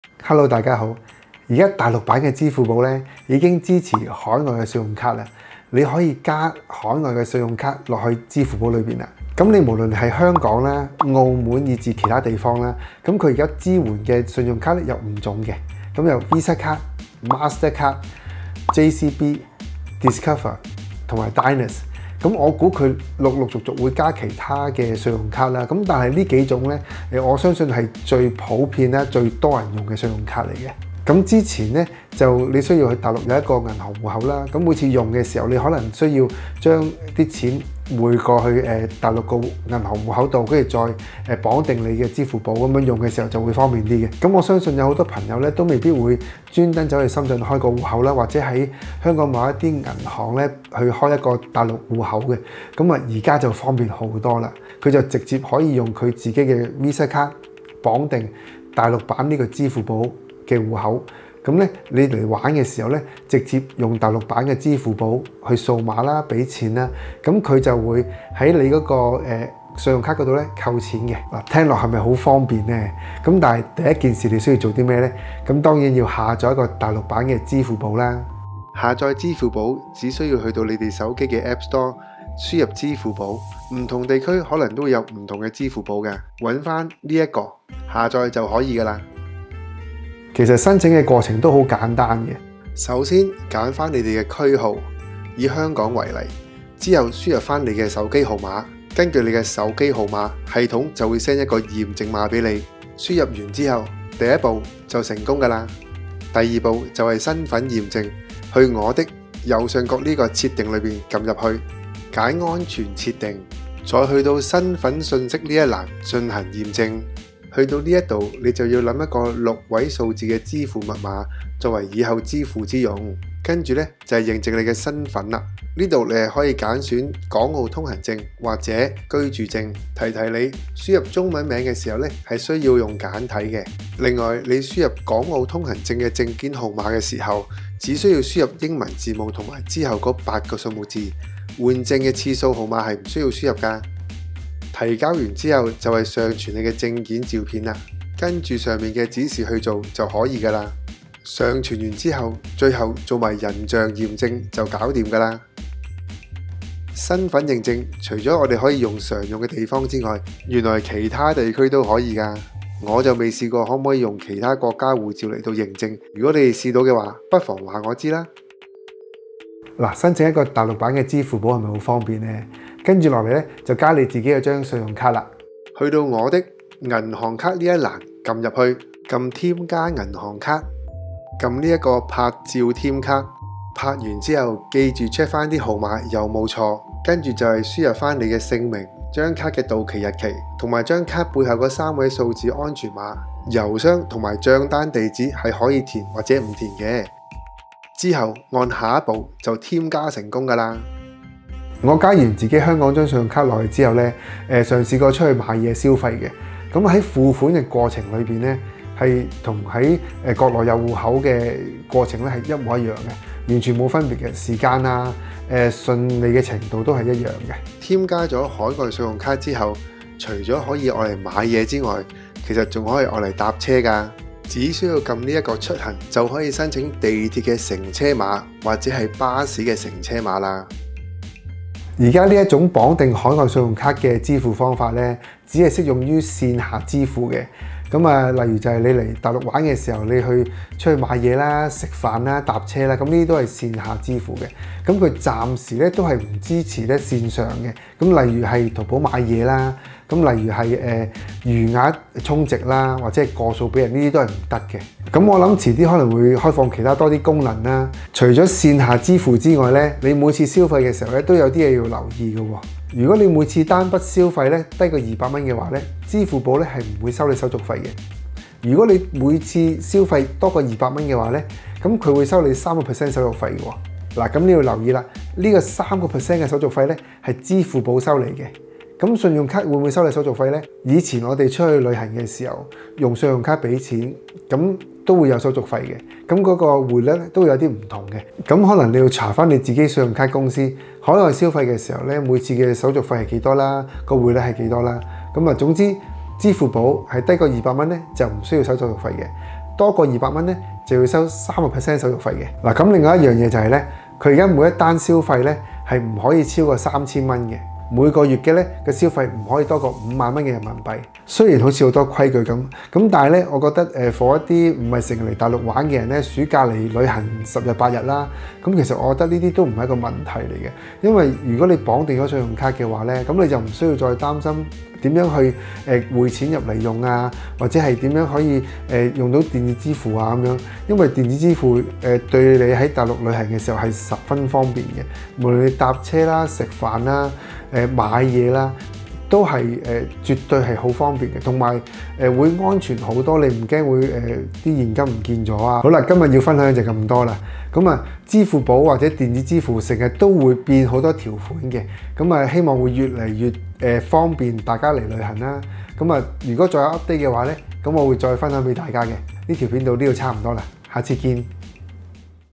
大陸支付寶終於可以綁定海外信用卡， 如何申請？點綁定？有無服務費？
最近大陸支付寶終於可以綁定海外信用卡， 大家來大陸旅遊就不用再煩惱如何用電子支付。

0:00 intro
1:18 下載大陸支付寶
1:32 申請支付寶及認證
2:49 綁定海外信用卡
3:21 使用支付寶
3:37 申請地鐵及公交乘車碼
3:51 注意事項
4:22 消費限額及手續費
5:45 outro